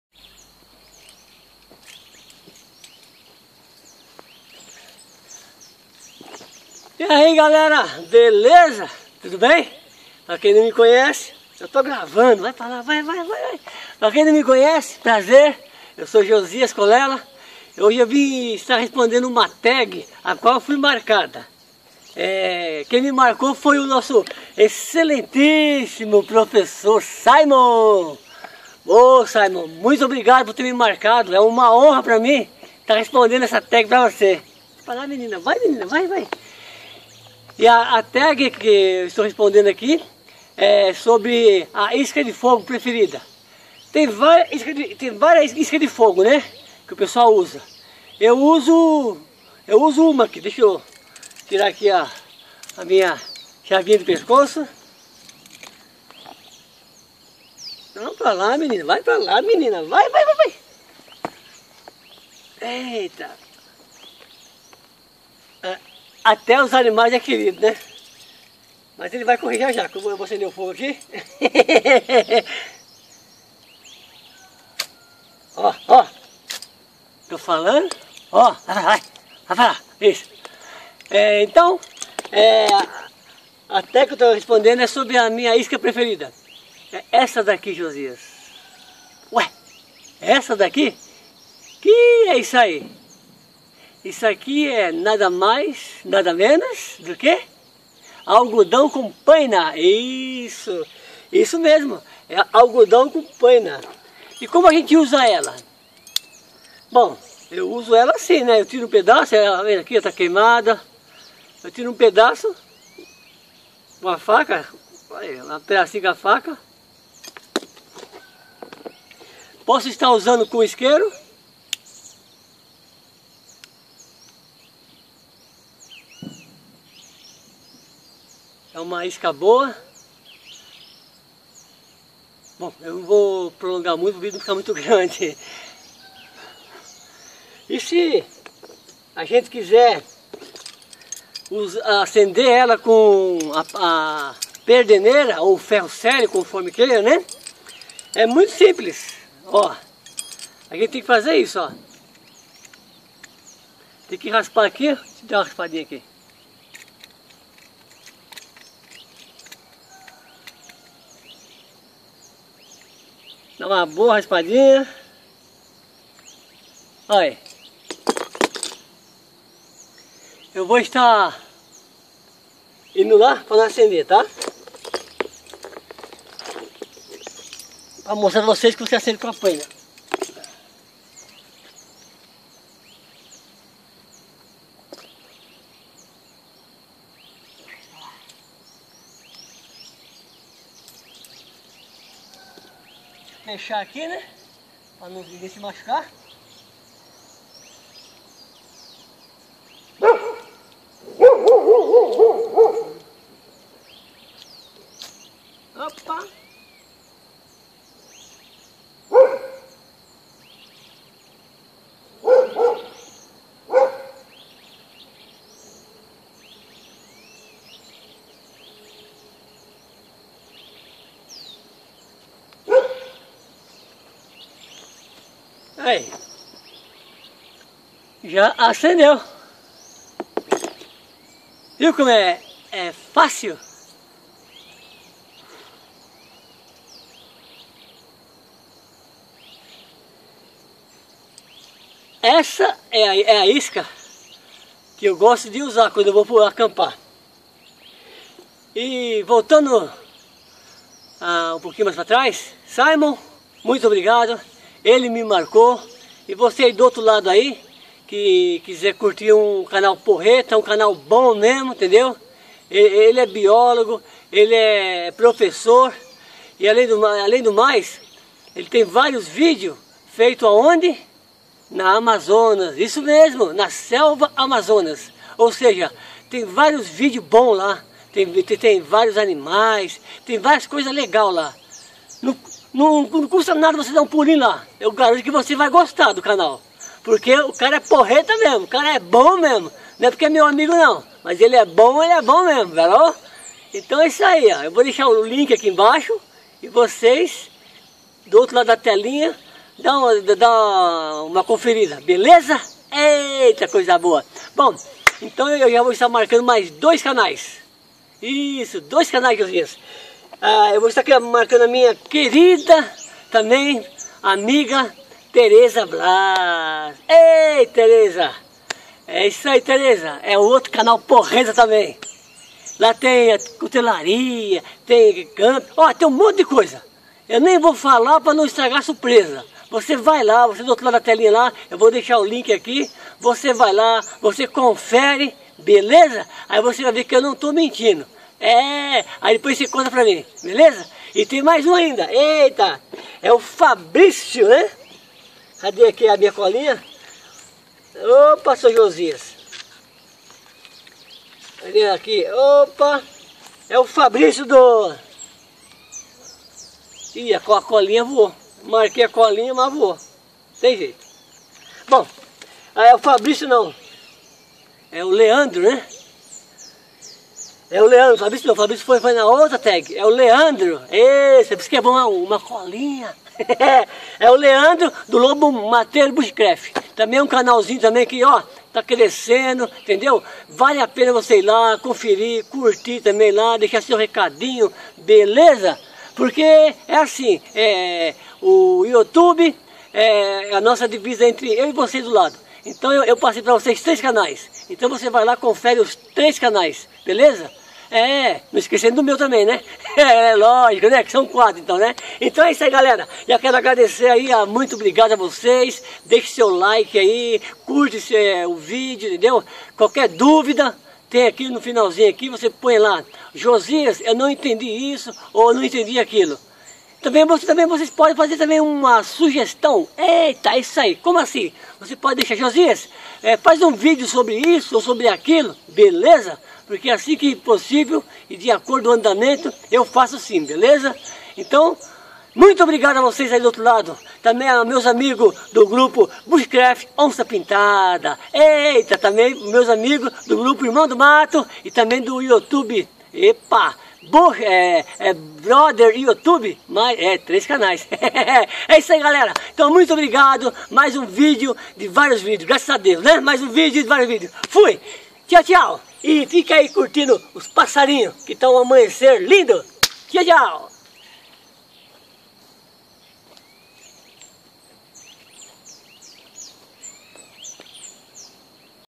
TAG Isca preferida para fazer fogo
este vídeo refere-se  a uma TAG que o Saymon me marcou  sobre a isca preferida  para fogo  vou está deixando o link do canal logo  abaixo

Vou está  marcando  mais  dois  canais para dar continuidade a  esta  TAG

Falha Técnica minha  o  certo  é   algodão de paineira com parafino  ou  vela   (  eu  falei algodão  com paina  faltou  a  parafina

Muito  obrigado  a  todos  um  forte  abraço